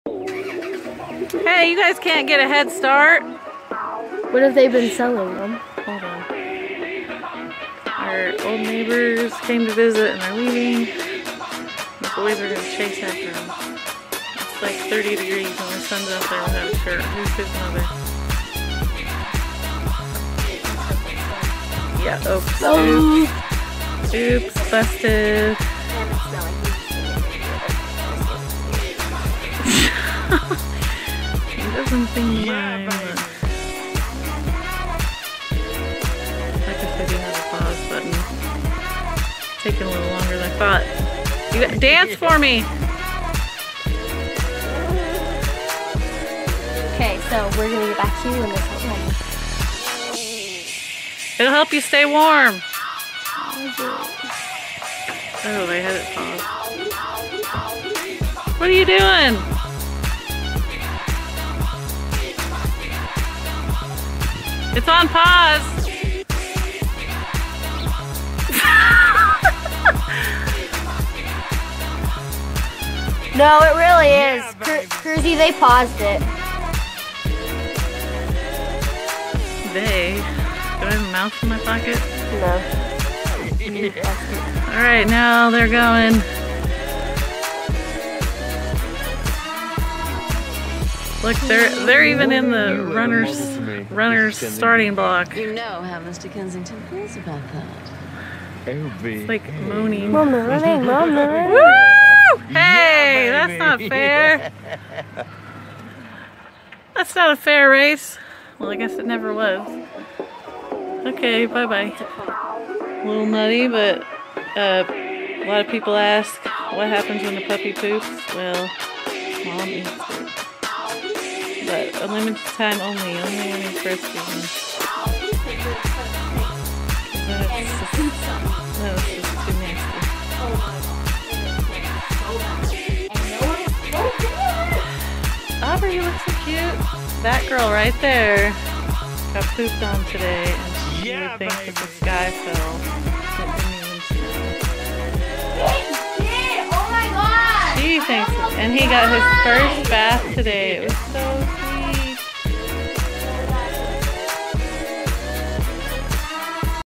Kids Race Friends Car. Momma Dog Licks Puppy's Poop while Breast Feeding!
Silly Times